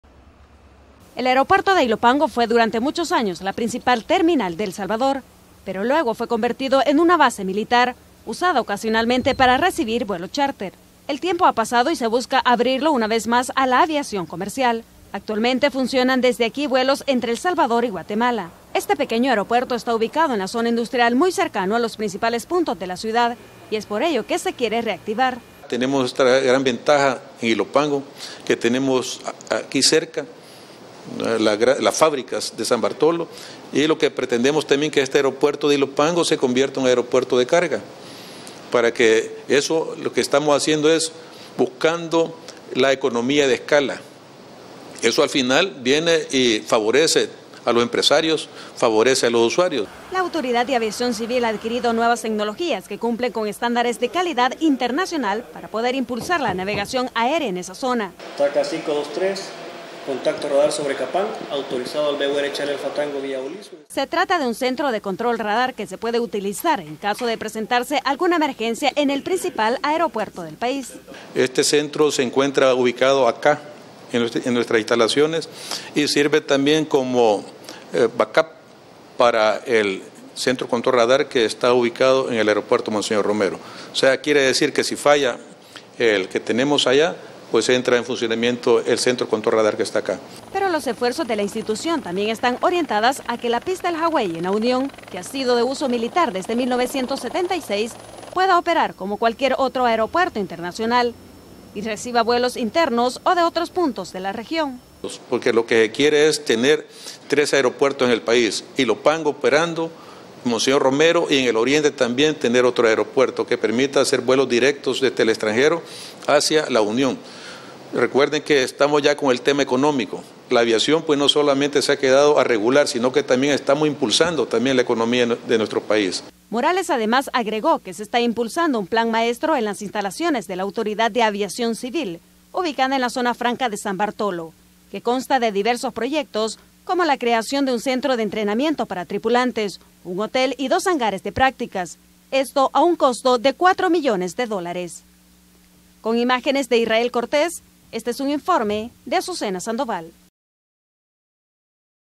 AVIACIÓN CIVIL IMPULSAN PROYECTOS
La terminal aérea de Ilopango está siendo dotada de nuevas tecnologías a fin de reactivar su funcionamiento, conozcamos más sobre este tema en el siguiente informe.